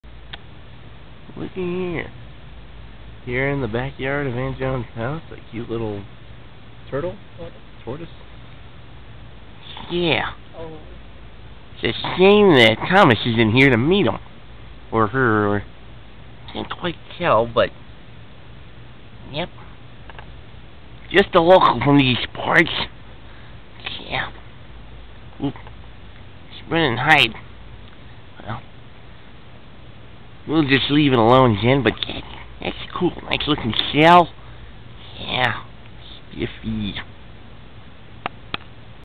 Alfred meets a real Tortoise
Visiting my aunt's house in Daytona Beach, we found a local tortoise right in the back yard.